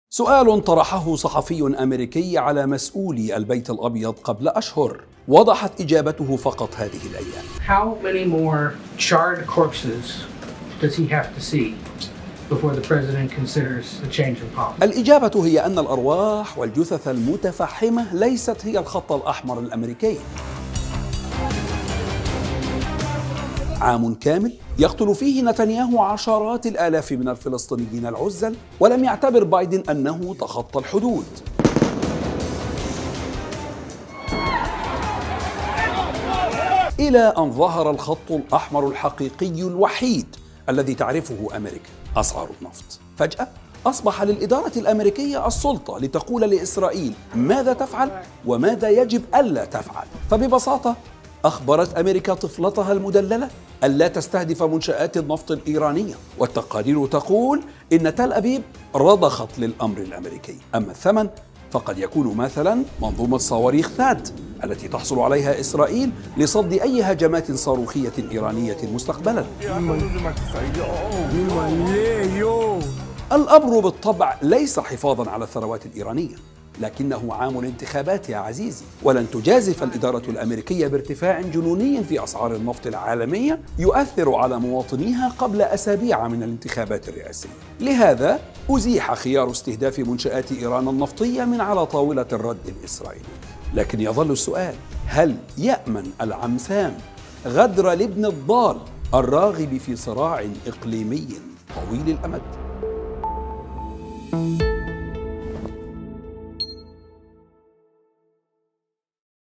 واشنطن تضع خطا أحمر لتل أبيب في الرد على إيران.. ماذا يكون؟
«لم يحركها إزهاق الأرواح ولا الدمـ.ـار» .. واشنطن تضع خطا أحمر لتل أبيب في الرد على إيران.. ماذا يكون؟